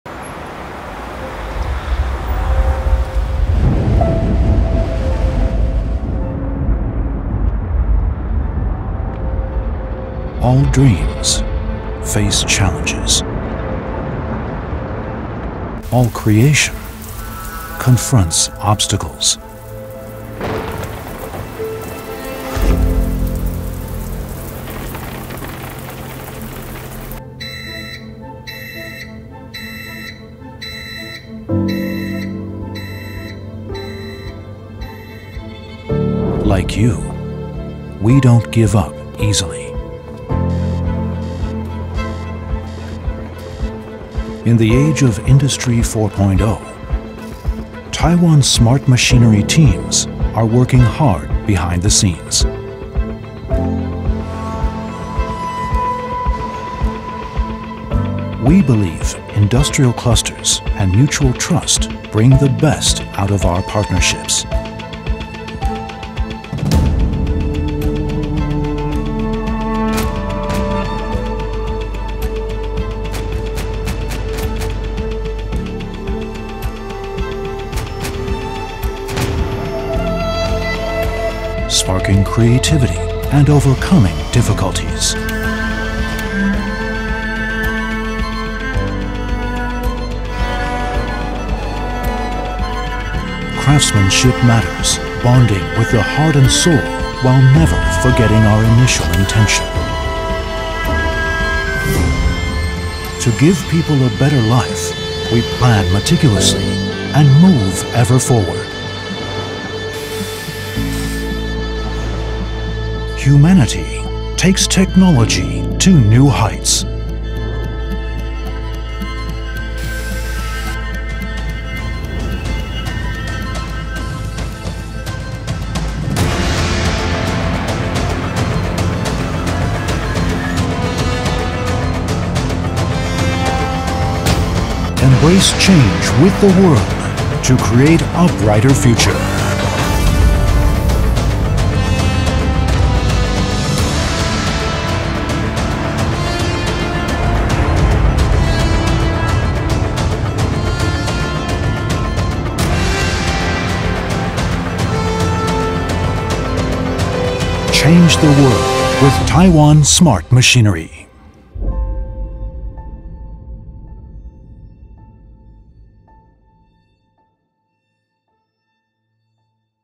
Change the World with Taiwan Smart Machinery | Taiwan Excellence台灣精品
Humanity takes technology to new heights – this is the belief of Taiwan's Smart Machinery teams.
In the age of Industry 4.0, we assist you for sparking creativities and overcoming difficulties.
With industry clusters and mutual trust,
we plan meticulously and move ever forward,
to let you embrace change with the world to create a brighter future,

CHANGE THE WORLD with TAIWAN SMART MACHINERY